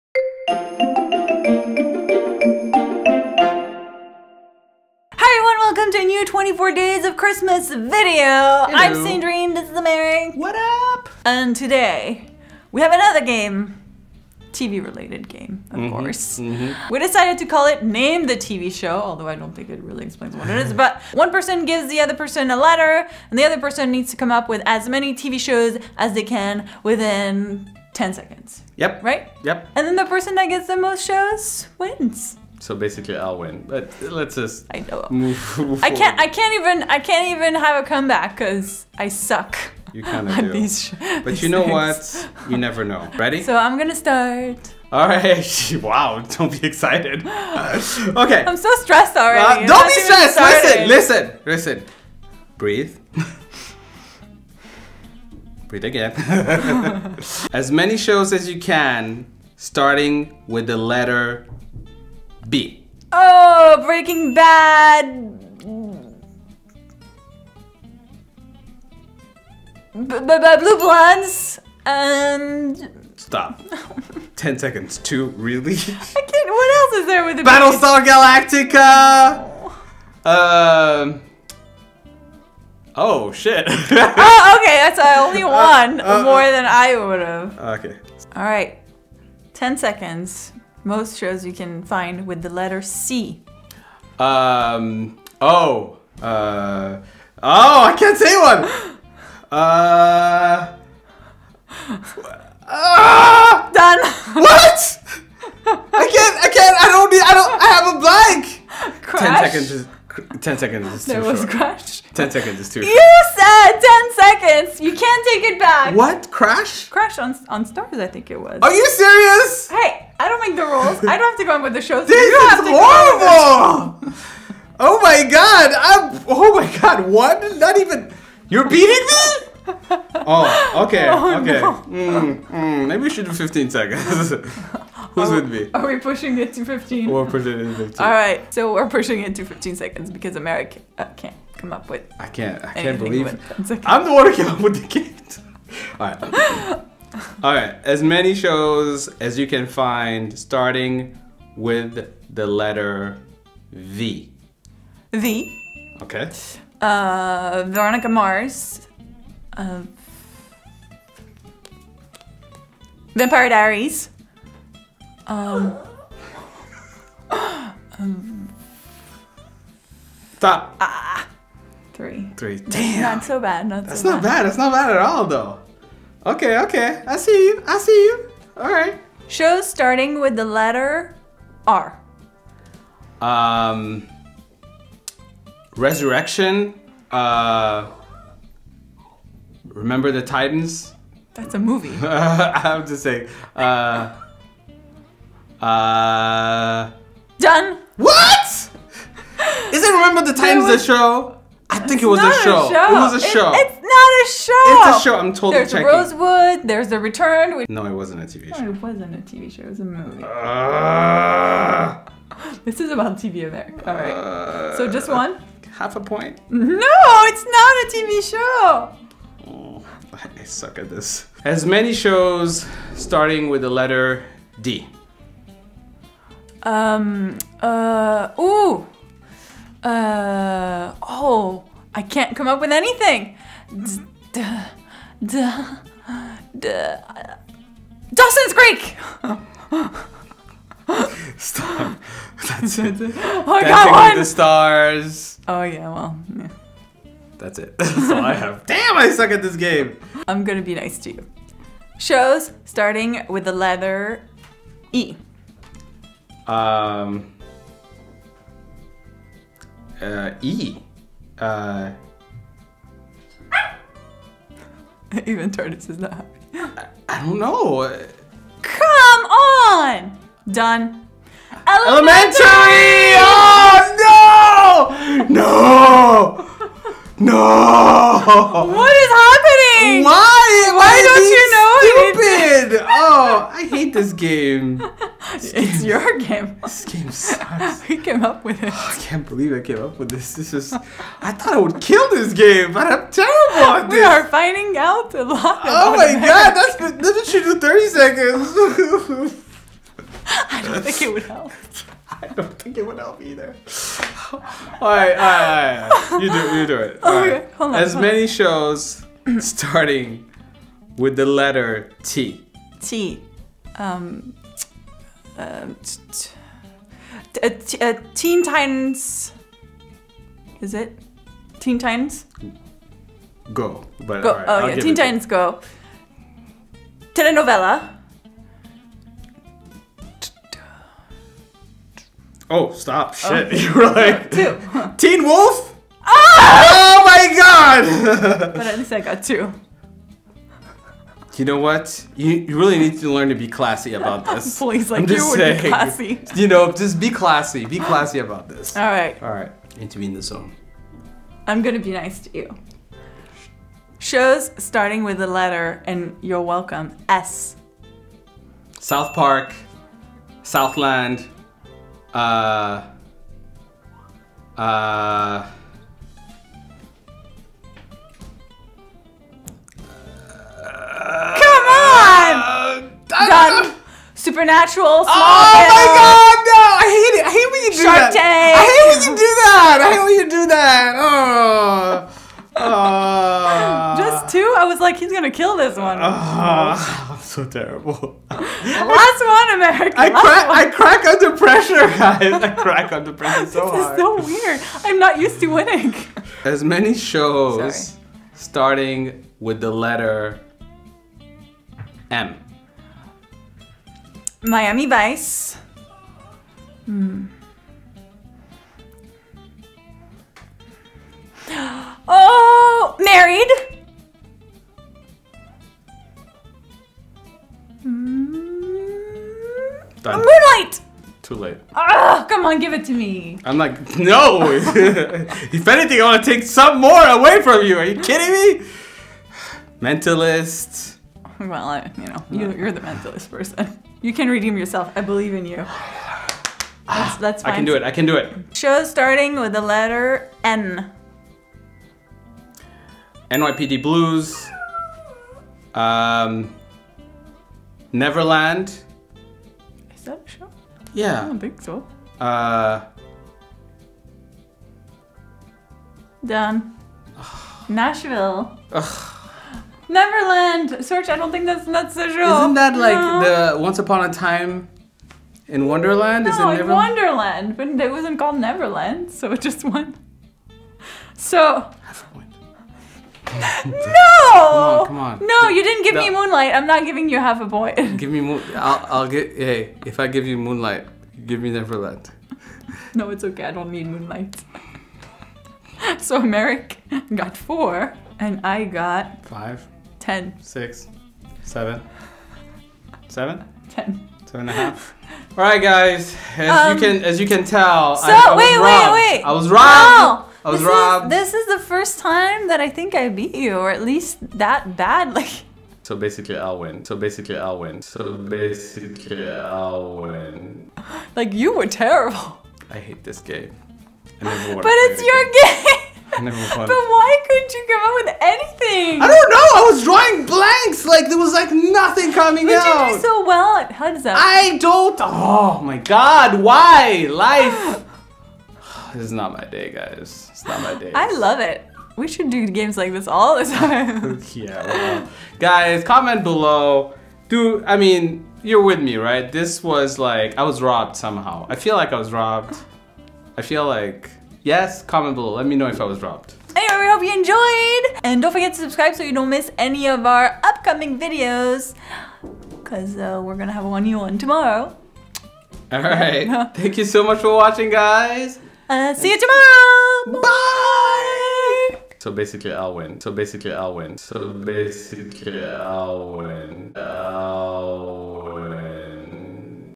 Name the TV Shows!
Welcome to Day 12 of 24 Days of Christmas on HappyCool!! We a new game, that we decided to call “Name the TV Shows!” (not crazy about that name to be honest! haha!) in which we need to come up with as many TV shows names that start with a specific letter. Things don’t turn out exactly as we thought they would...

Send us things!

HappyCool
13636 Ventura Blvd #398